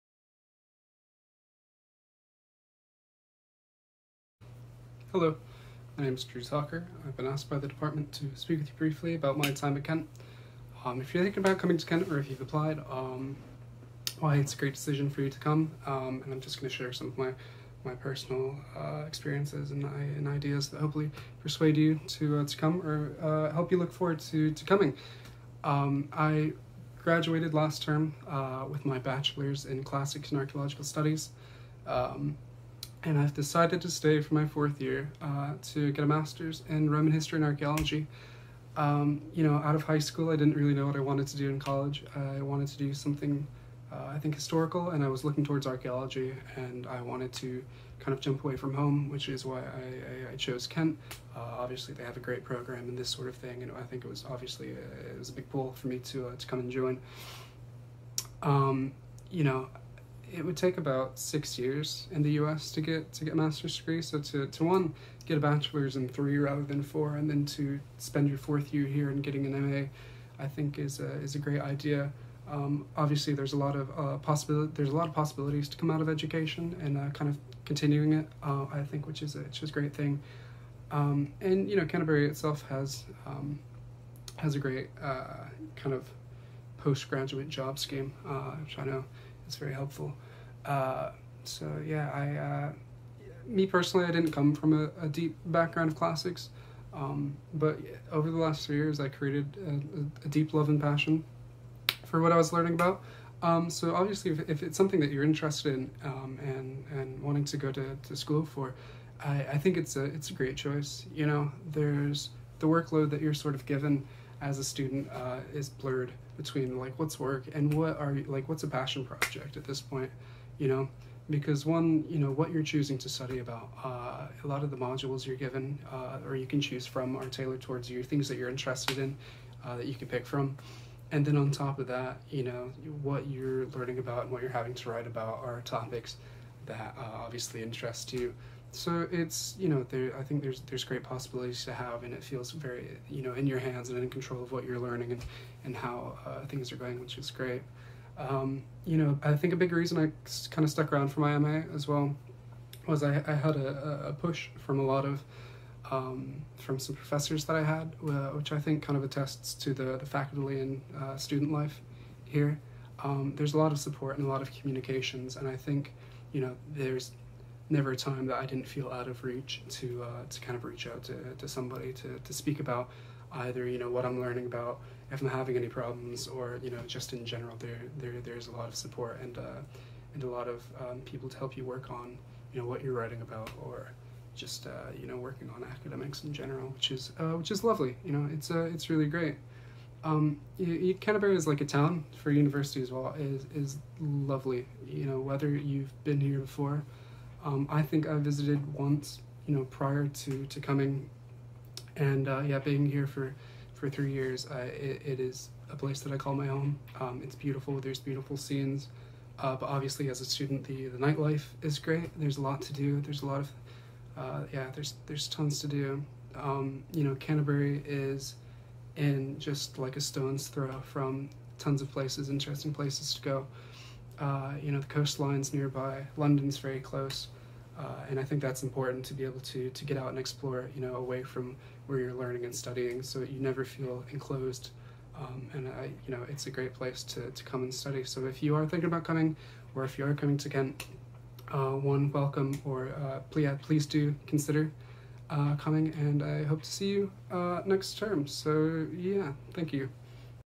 Why choose Kent for Classical and Archaeological Studies? Drew Tucker explains why he's glad he did!
Drew takes us through his reasons for coming from the US to study at Kent , explains the things he enjoyed about the degree, and wraps up with a list of good reasons to live and study in Canterbury.